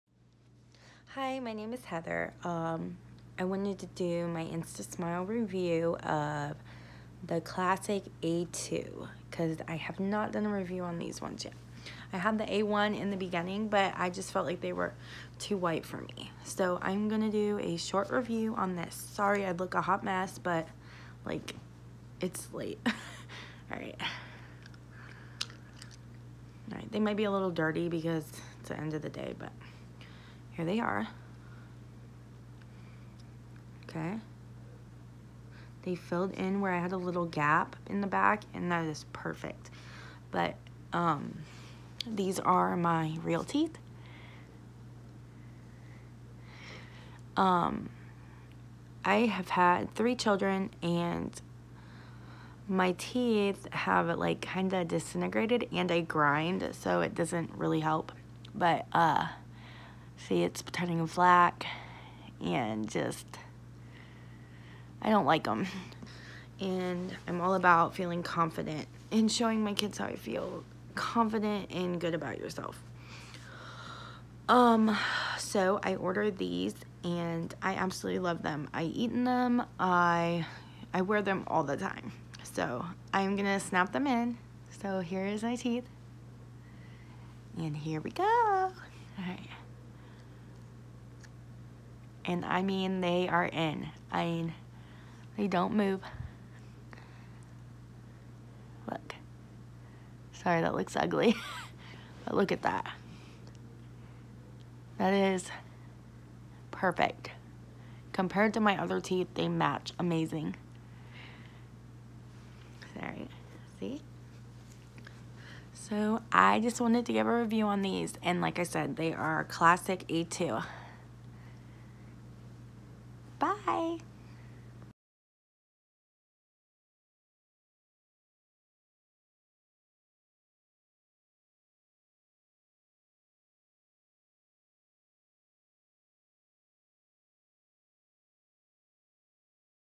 Smile Makeover After Pregnancy | Clip On Veneers | Instasmile Review
instasmile Clip On Veneers review - instasmile is a revolutionary, and affordable option to cover crooked, chipped, missing and discoloured teeth.

Heather found instasmile as a cost-effective alternative to give her the ultimate smile makeover! After having three children, Heather’s teeth become brittle and coupled with her bad habit of grinding her teeth, her smile has slowly deteriorated.

Heather couldn’t believe her luck after finding out instasmile could transform her smile at a fraction of the cost of cosmetic dentistry, meaning it was affordable for her as a mother. Heather now feels so much more confident!

Once clipped in, Heather’s instasmile clip on veneers fit perfect, due to it being custom made to fit her after taking Dental Impressions of her teeth and sending these back to instasmile HQ to manufacture her smile. Unlike a ‘one size fits all’ snap on veneer such as Perfect Smile veneers, or Instant Smile which usually retails at a shockingly low price of £10/$10 and is an off the shelf product, which is not personalised.

Heather is wearing the Classic Top set instasmile in the shade A2 to colour match with her natural Bottom set of teeth.

You can watch Heather’s other review of instasmile, comparing the Straighter and Natural instasmile design,